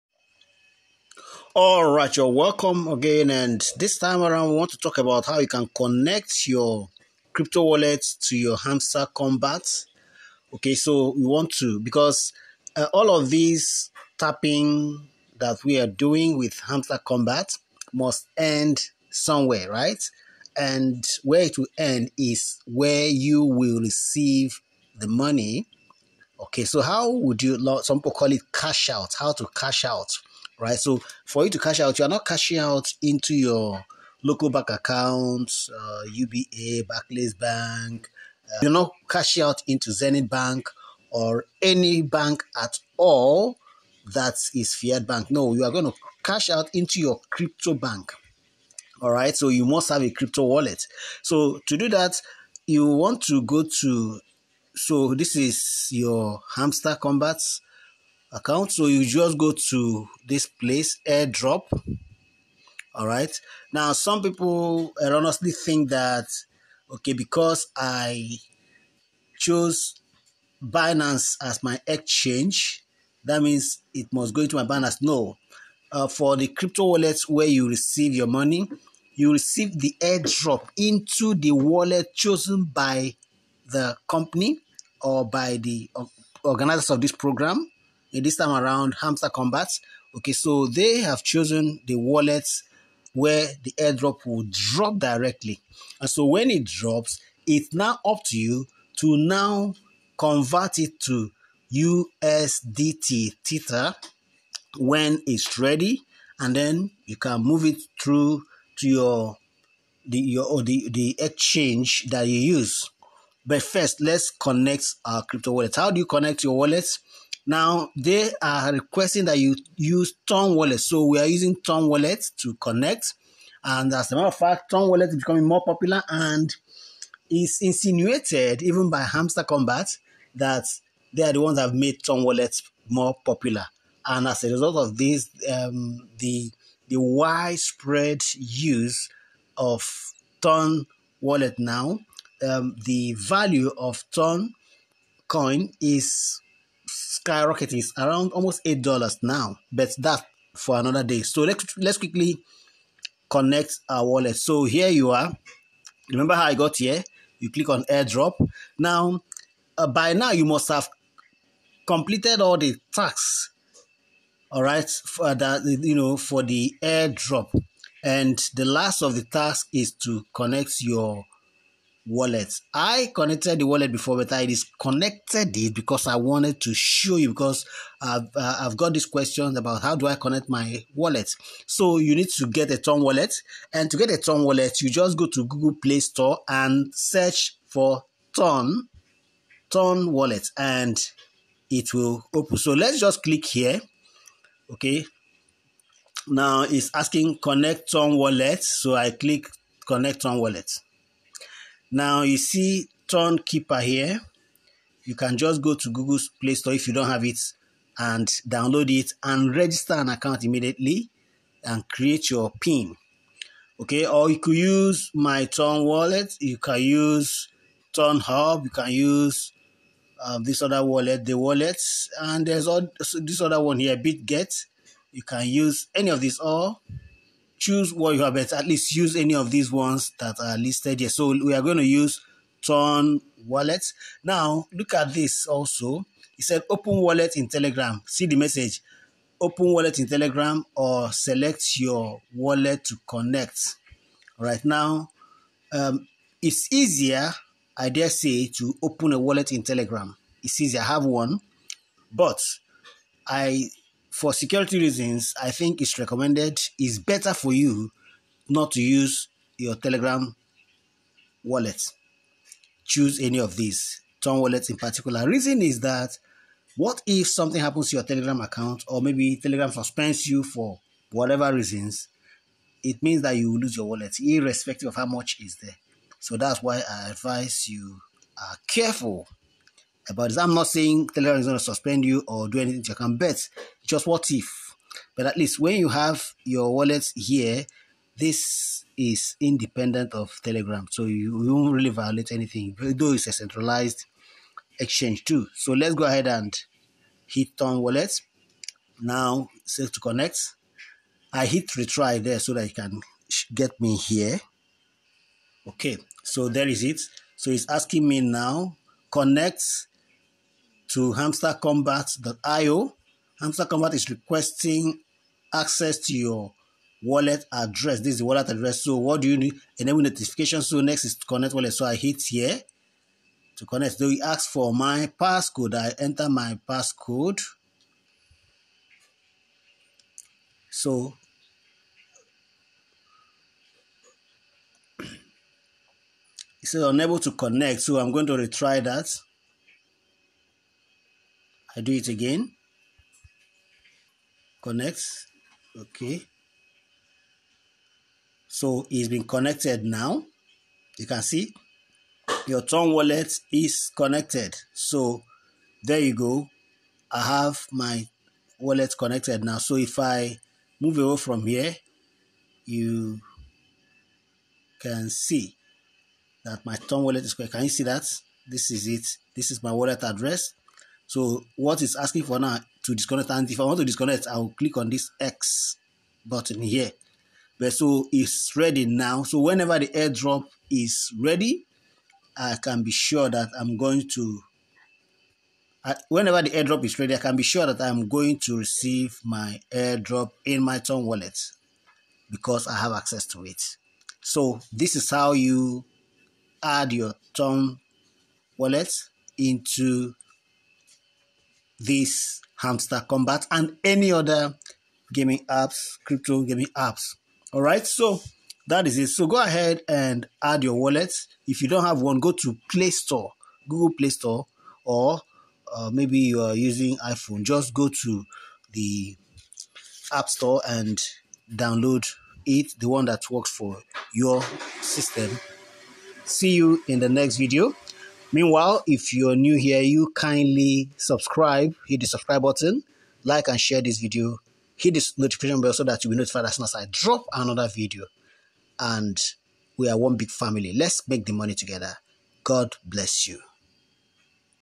HOW TO ADD YOUR CRYPTO WALLET TO YOUR HAMSTER KOMBAT
Learn how to how to add your Crypto Wallet to your Hamster Kombat.
Watch to the end to what to do.